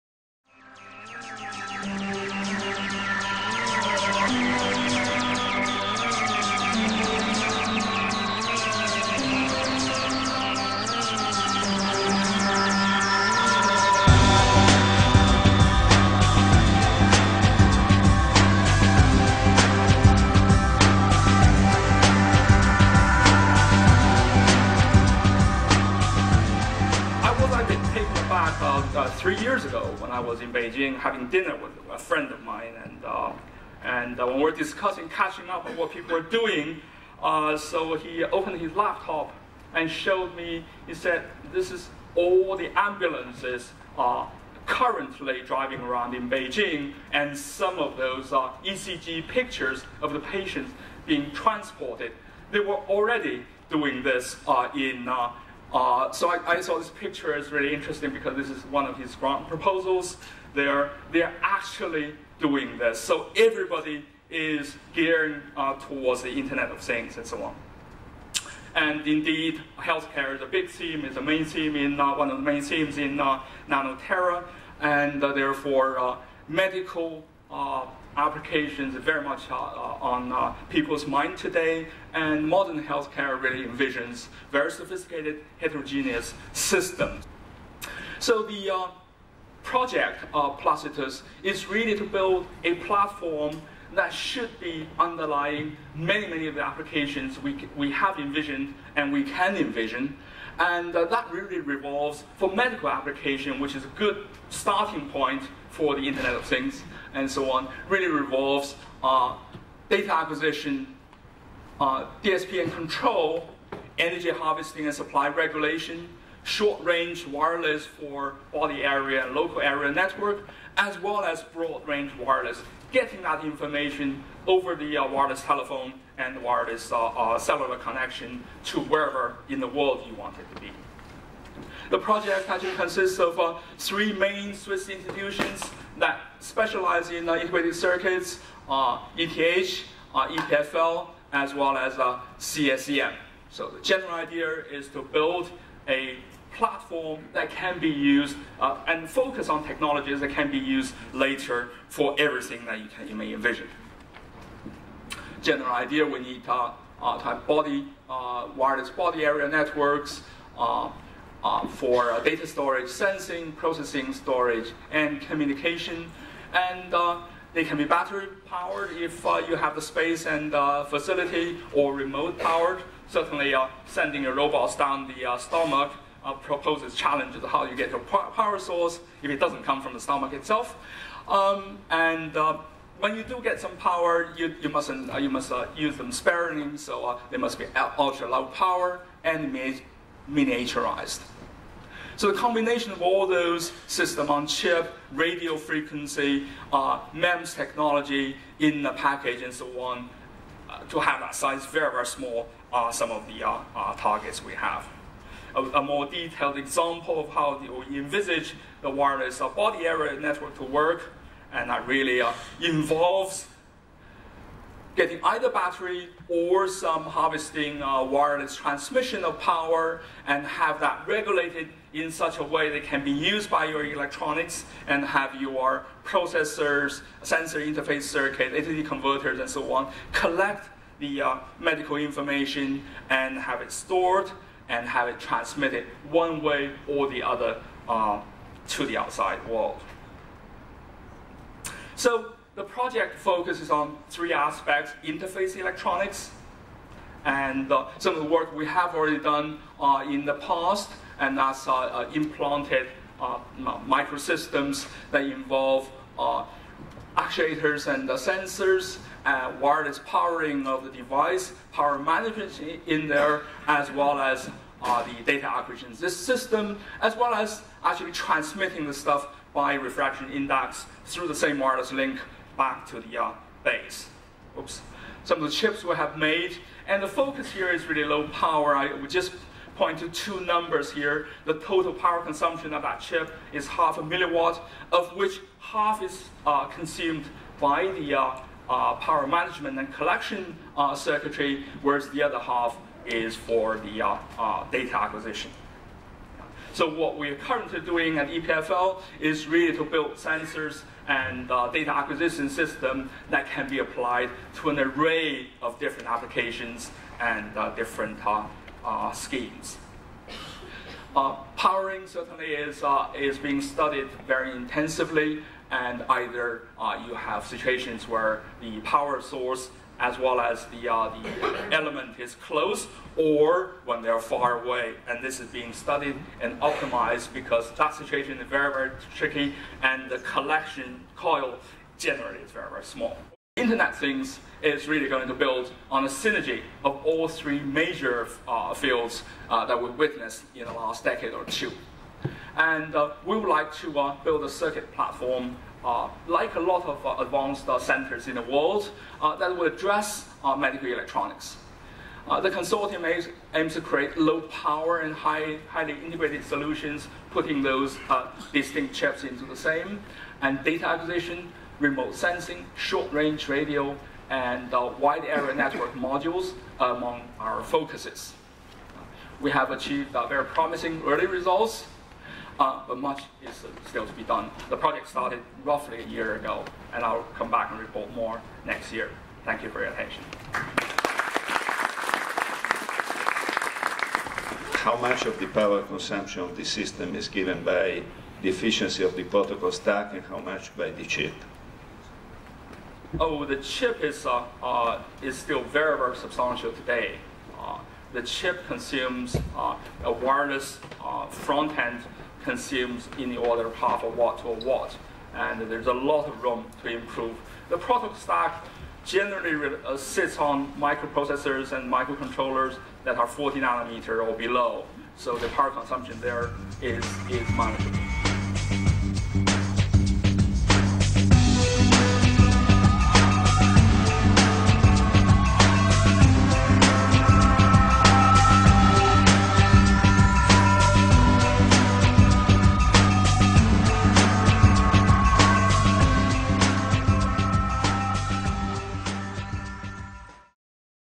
Nano-Tera: PlaCiTUS (2011)
Qiuting Huang of ETHZ/Integrated Systems Laboratory
The revolution in information and communication technology (ICT) that is taking information flow into the era of tera-bit/s and the biomedical advances down to molecular scale would not have taken place without the accompanying downscaling of CMOS technology to the nano scale device size and tera system complexity. Applications in the field of health, security and environment can clearly take full advantage of this on-going revolution. Access to nano-scale CMOS technologies and know how in the design and production of sophisticated circuits and systems needed for health care, ambient monitoring and based on such technology are vital to Switzerland for retaining and expanding its knowledge-based, industry. In this context, this project will develop a nano-CMOS design platform underlying most health, security and environment applications.

In addition to the demonstrators planned for this project, most nano sensor and actuator projects undertaken within the nano-tera initiative will require sufficiently sophisticated and integrated circuits to support their functionalities. Instead of diffusing such work into each such project, the current project assembles the best integrated circuit and system experts in Switzerland and address the underlying circuit aspects of nano-systems of heterogeneous nature, that combine novel nano sensing and actuating devices with nano-CMOS circuits and systems of commensurate functionalities and complexity to offer the sophistication and meet the many challenges of the 21st century.

May 2011, full presentation length: 14 minutes 50 seconds